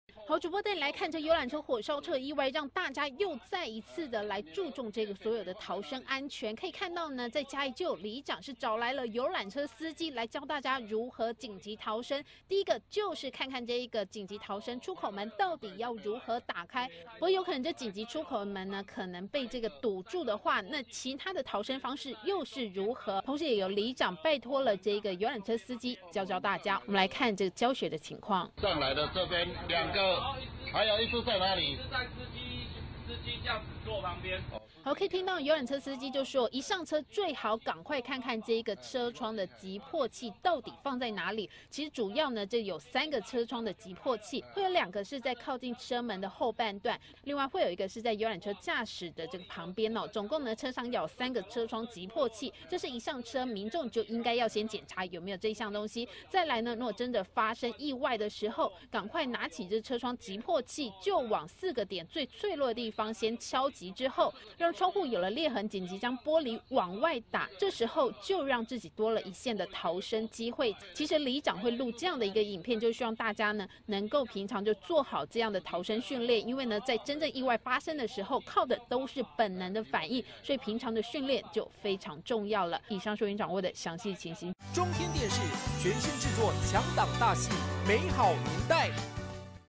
【即時】遊覽車火燒怎逃 里長拍影片教逃生
遊覽車的火燒車意外，讓大家又再一次的注意逃生問題，嘉義就有里長特別找來遊覽車司機拍攝影片，提醒大家逃生要訣。